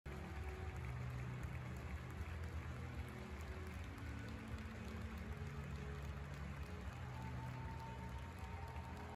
Pitt basketball honors Damar Hamlin during game
University of Pittsburgh basketball team took pause on Tuesday night during their game against the UVA Cavaliers to honor Pitt alum Damar Hamlin, who suffered cardiac arrest during the Monday Night Football game between Bills and Bengals.

Hamlin is a native of Pittsburgh and played football for the University of Pittsburgh from 2016 to 2020. The latest updates from